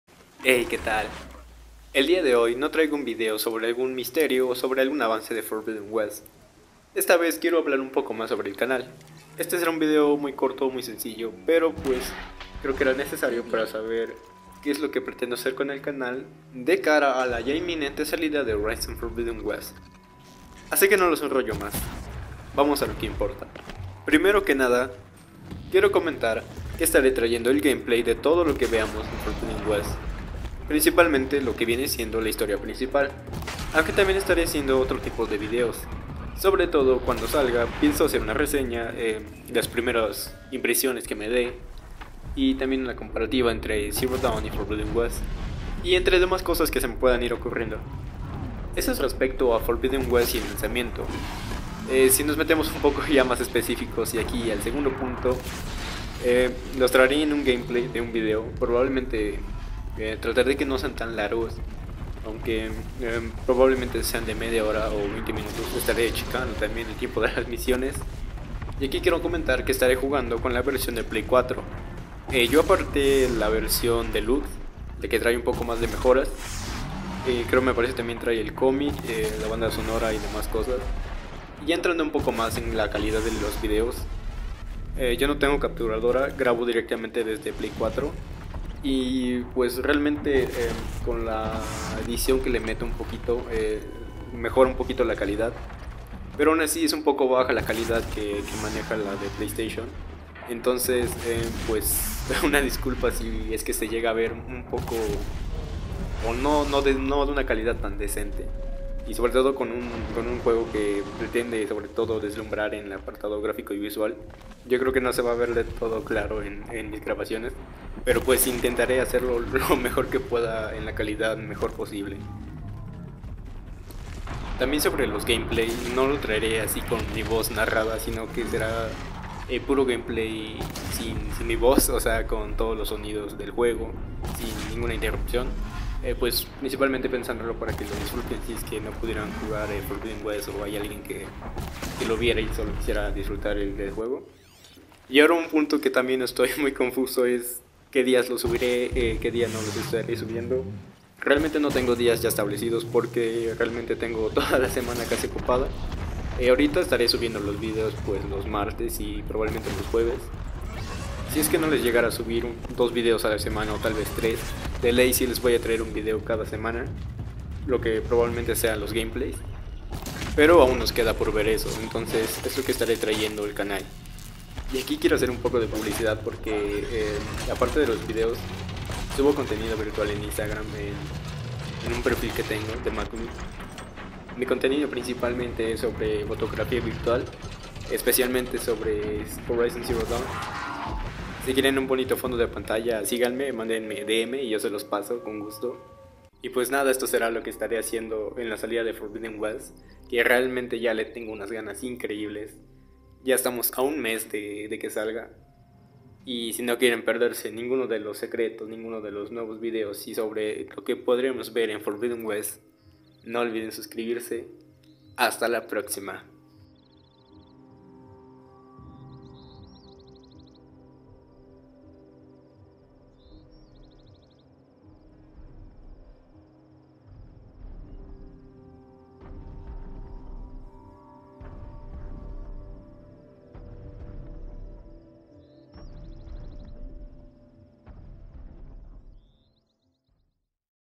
¿QUE PASARA CON EL CANAL CON LA SALIDA DE HORIZON FORBIDDEN WEST?
Hey que tal, esta vez les traigo un video no mas que nada para comunicar como será el contenido que estaré subiendo ante la salida de Horizon Forbidden West. No se preocupen que en unos dias subire un video donde veremos que nos podemos esperar de Horizon Forbidden West c;

Tkm